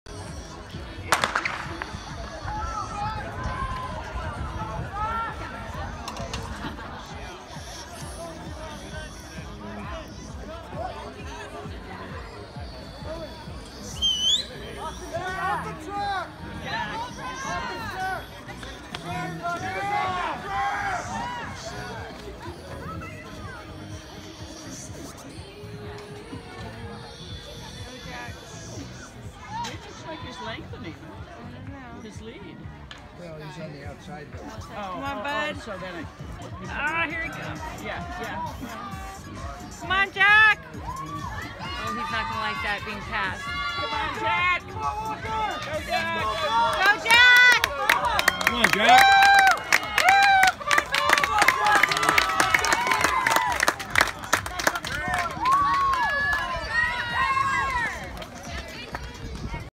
Jack Hildebrand Intrasquad Time Trials 400m on February 29th, 2020
Jack Hildebrand is in Lane 6. Recorded time was a 55.88.